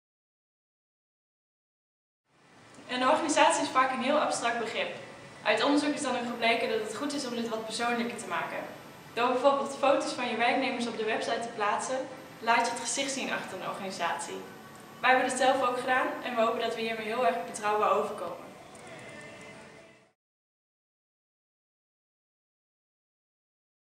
Communicatietip week 48
Geef je organisatie een gezicht. Laat je medewerkers op je website zien.